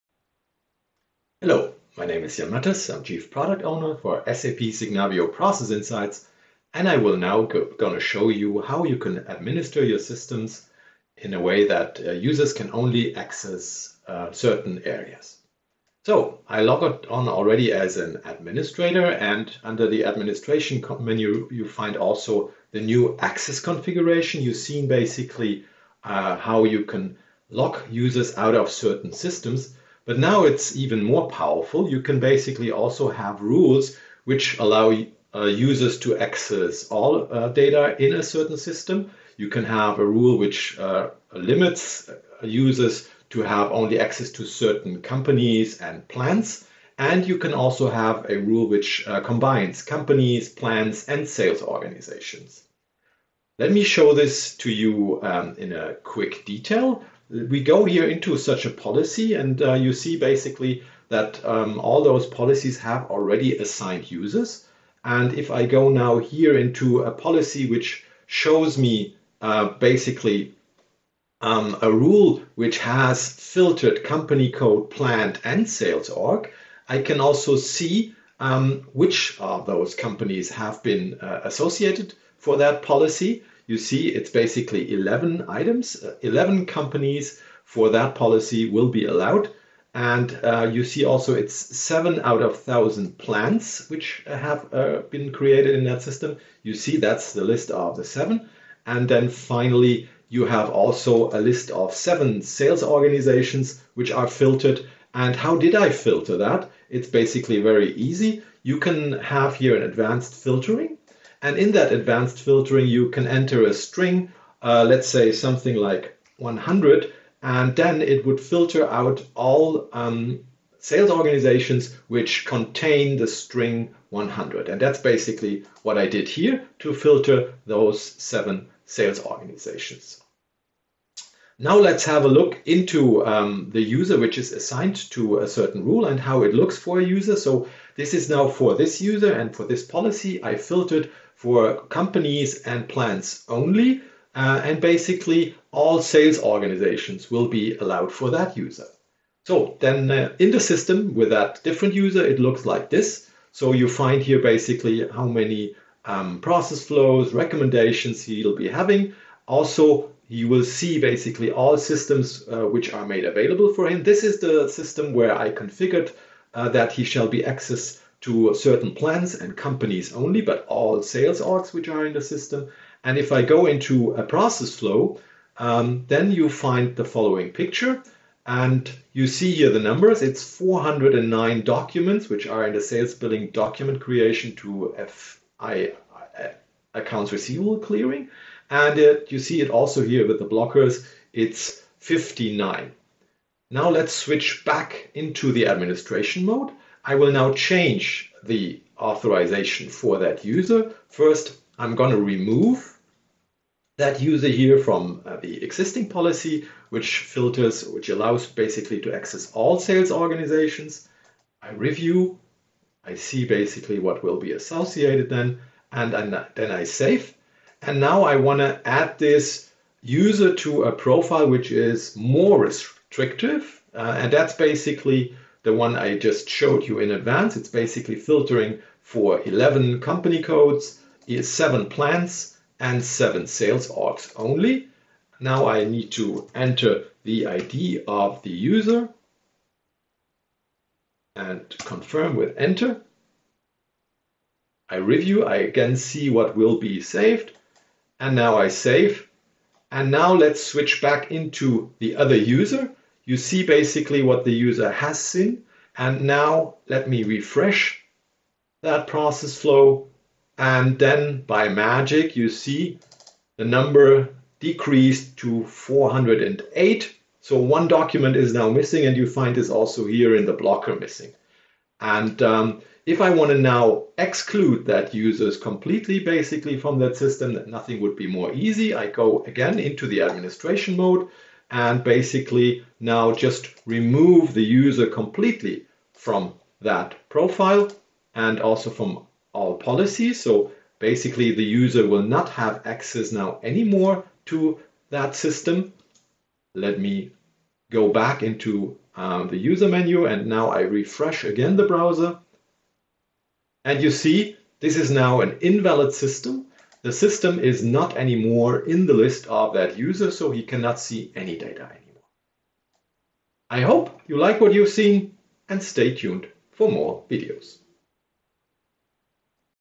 SAP Signavio Process Insights Access Management
Demo and tutorial on how to configure user authorizations for systems, companies, plants and sales organizations in Signavio Process Insights